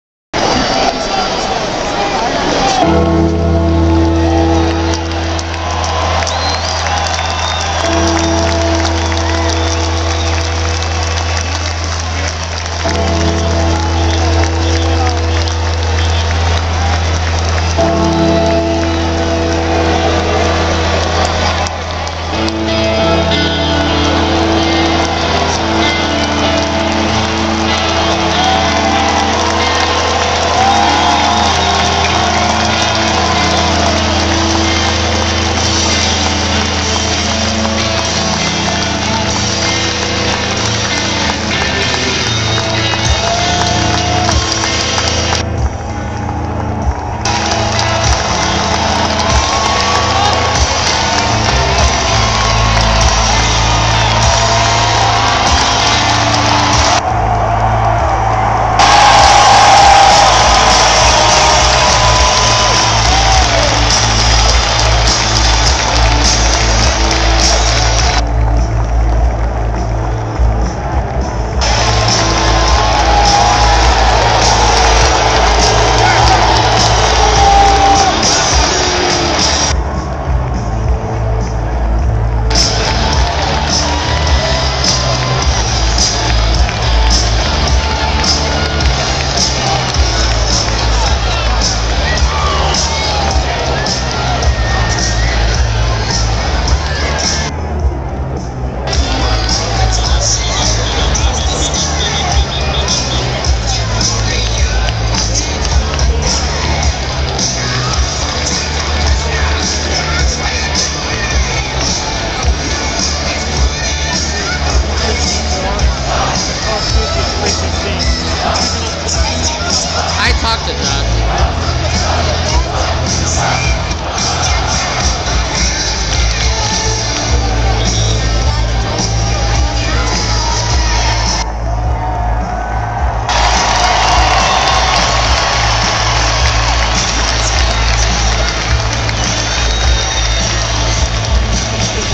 Ohio State Buckeyes Take The Field
The 2009 Ohio State Buckeye Football Team Takes The Field During the 2009 Ohio State Vs. Illinois Game.

As They Run Out Of the Tunnel They Preform "The Hive"

Tressel brought to the Buckeye football program two pre-game traditions he developed at Youngstown State. Prior to its warmup routine before every football game, the team exits the locker room as a unit in a controlled manner, linked arm-in-arm in a group known as "The Hive". After warmups the team returns to the locker room, and when it next appears, runs onto the field and forms a circle of players around the strength coach, who exhorts the team into a frenzy in which they pummel each other with fists.